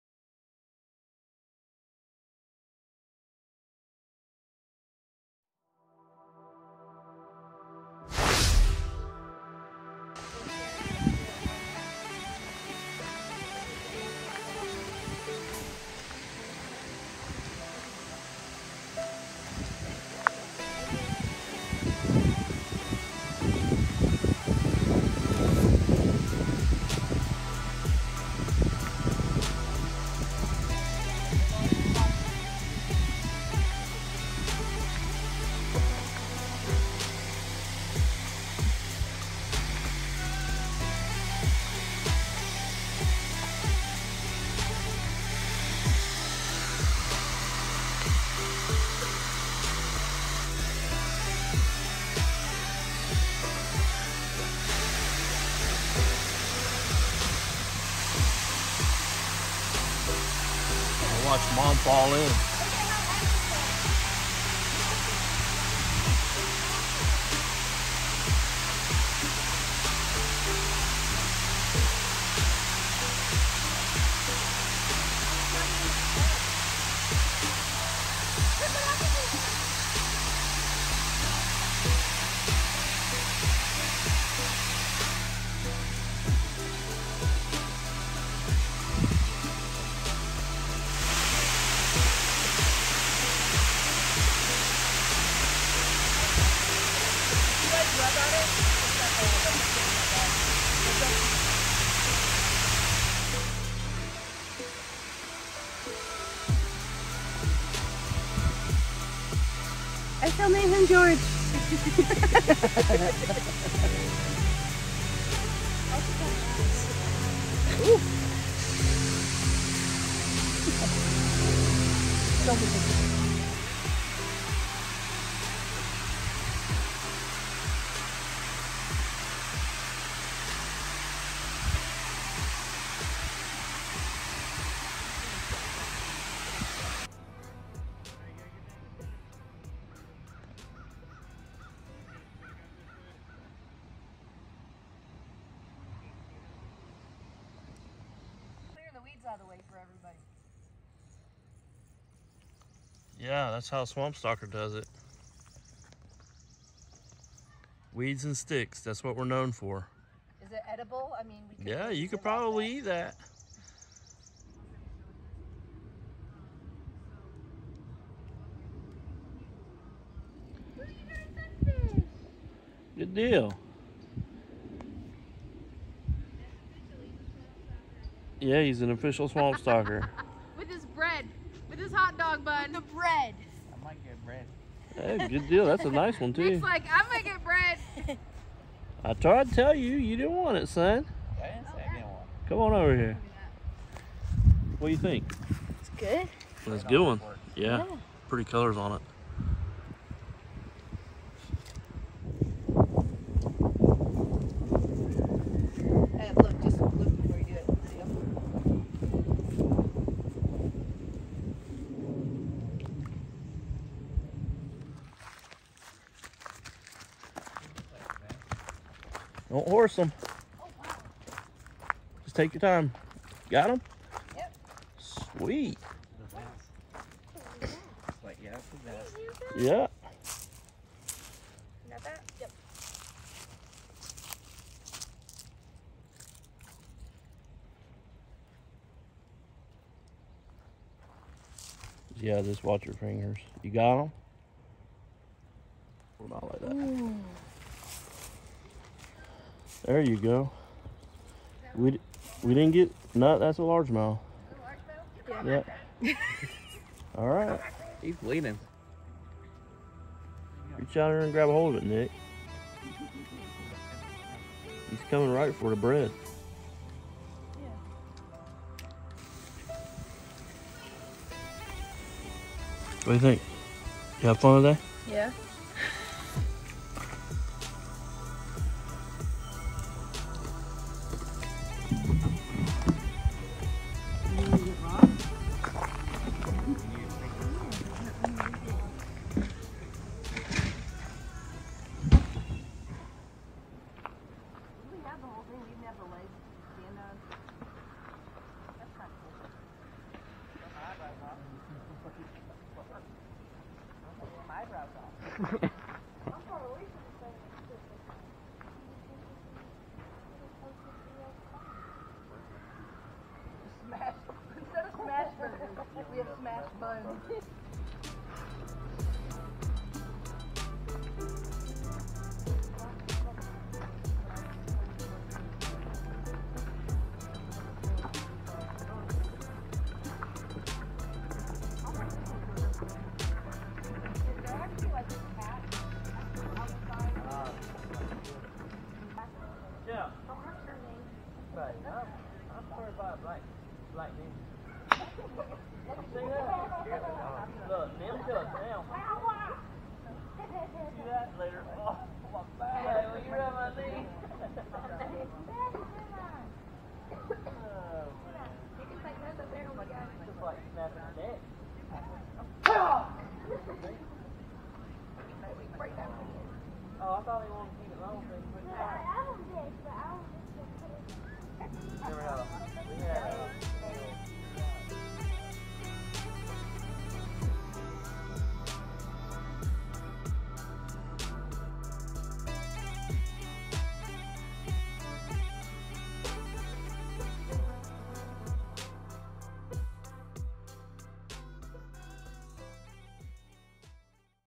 Spring Camping at Kings Mountain State Park, SC Part 2
On this week’s episode of Swamp Stalker Outdoors we travel to Kings Mountain State Park in South Carolina to do a little weekend camping and collaboration with YouTube channels Earlybird Farm, SC and Dean Family Acres. We also have a couple special guests come along and camp with the Swamp Stalker Outdoors crew. Be sure to check out the links below to see each of their channels and their videos from this trip.

Spring Camping at Kings Mountain State Park
(Part 1)👇